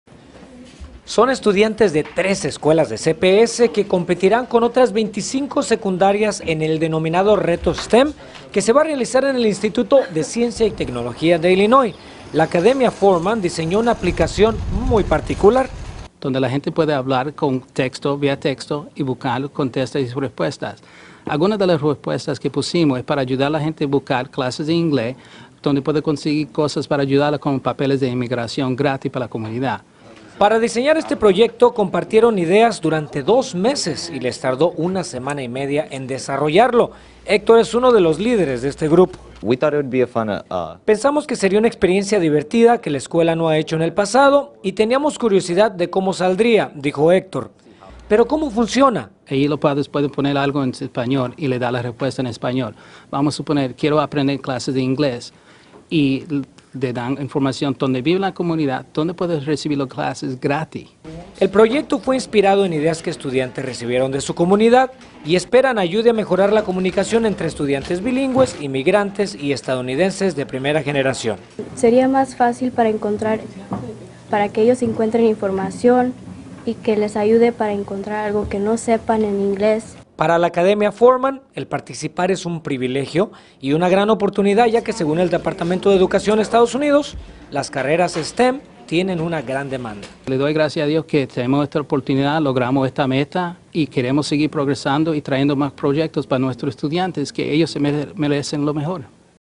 Estudiantes de Chicago listos para 'Reto STEM'
Jóvenes de la Academia Foreman diseñaron una aplicación que brinda información en español a todos los que estén interesados en recibir, por ejemplo, clases de inglés gratuitas.

{url}